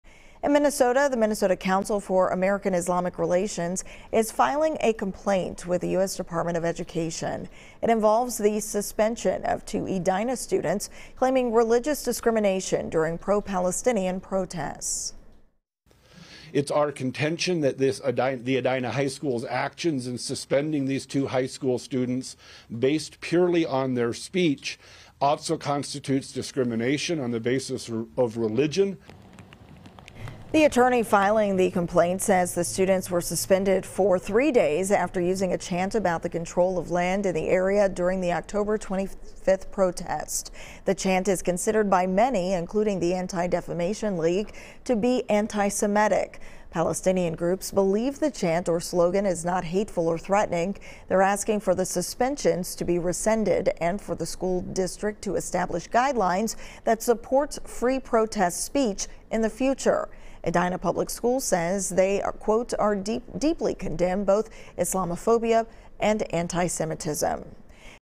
CAIR-MN says Edina students' suspension over pro-Palestinian chant should be rescinded
The Minnesota Council for American-Islamic Relations is filing a complaint with the U.S. Department of Education over the suspension of two Edina students, claiming religious discrimination during the pro-Palestinian protests.